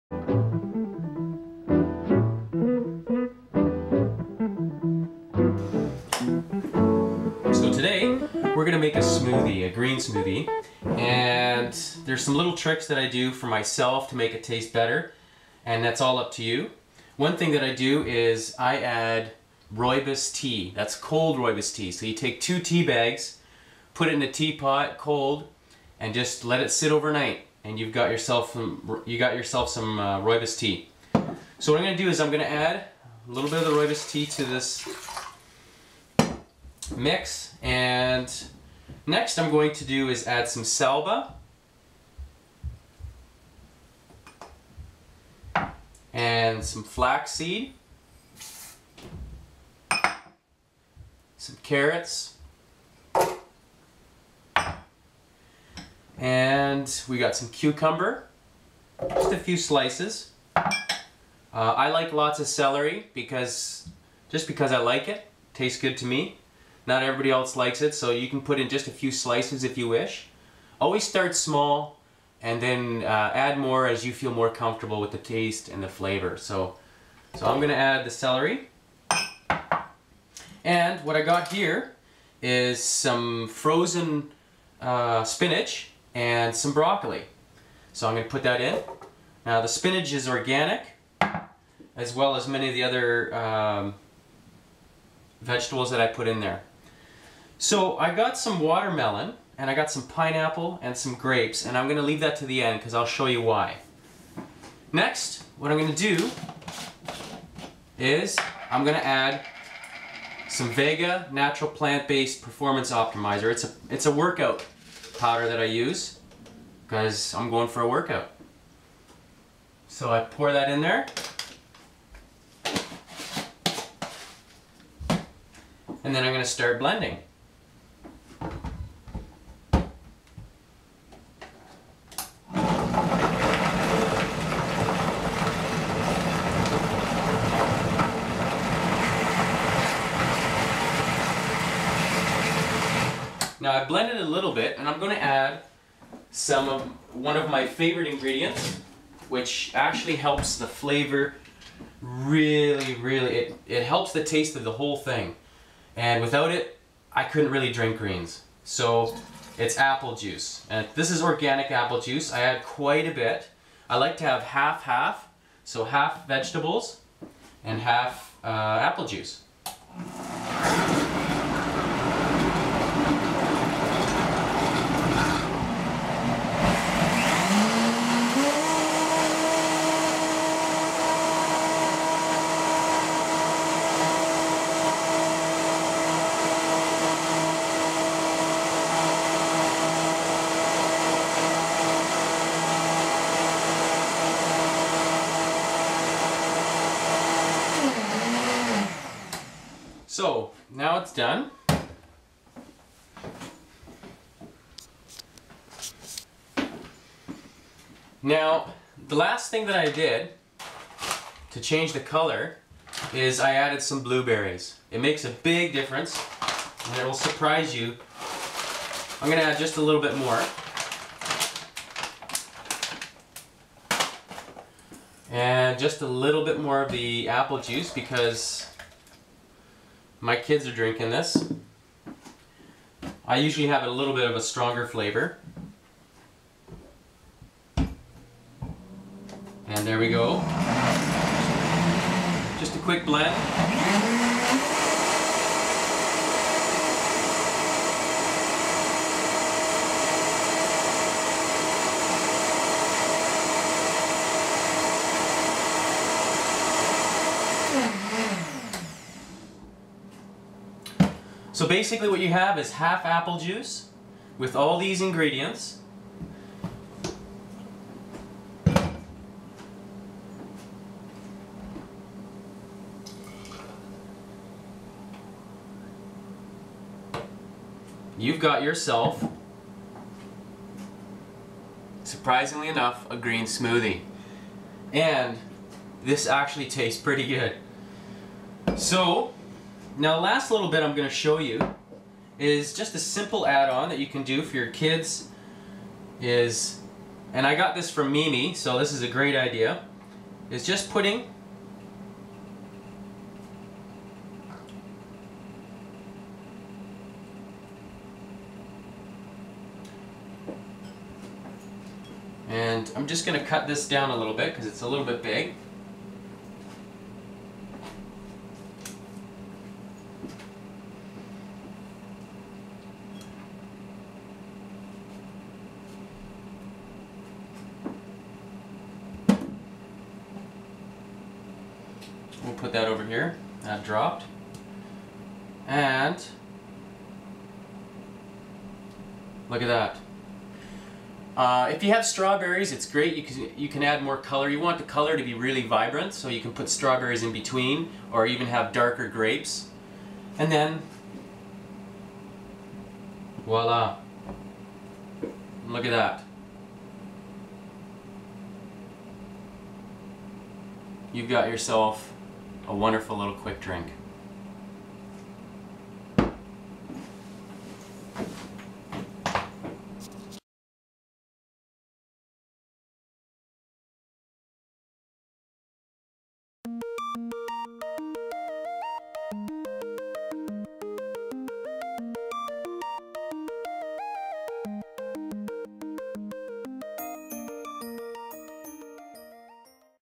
Green Smoothie SURPRISE
This is a smoothie to die for! It is packed with greens and has a strong hint of sweet apple. Shed pounds and give yourself a brain wash!!  ;) I use this for my heavy training and I highly recommend it. A great way to start your day, plus you are fighting cancer and feeling great while doing it.